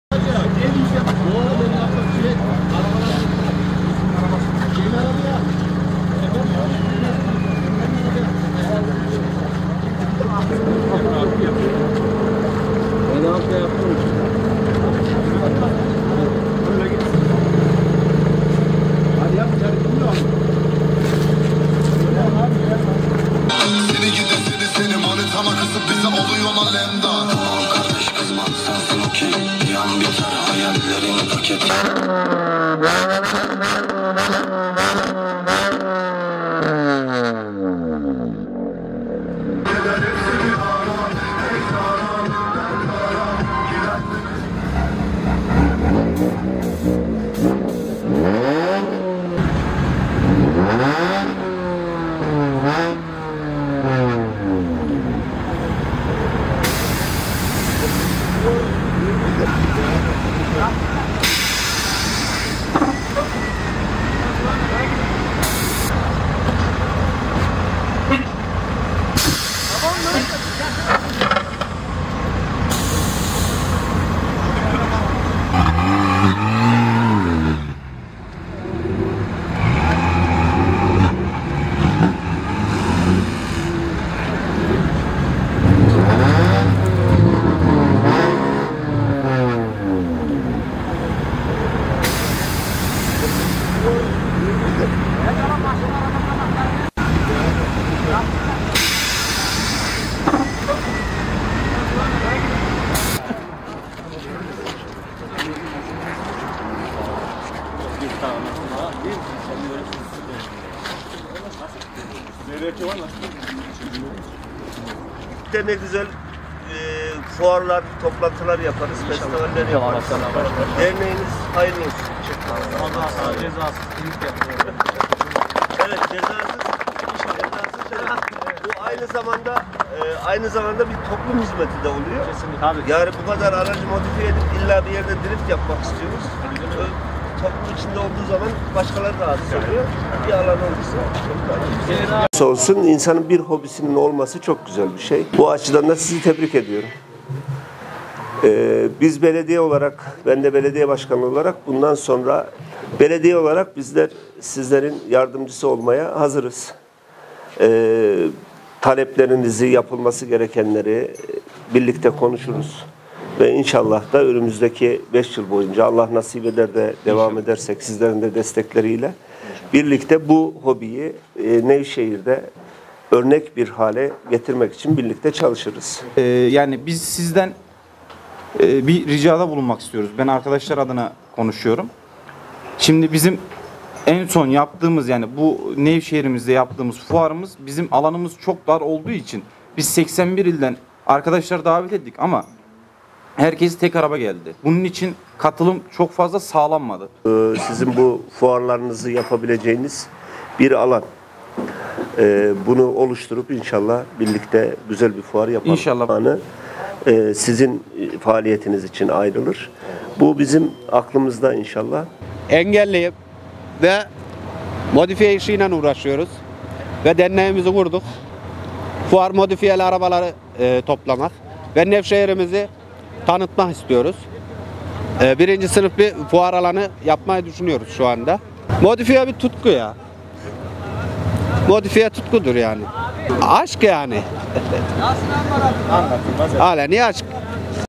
Nevşehir'de modifiyeli araç tutkunları dernekleşti
Nevşehir'de Modifiyeli Araç Tutkunları Dernekleşiyor: 'NEVFEST Otomobil Sevenler Derneği' Açıldı.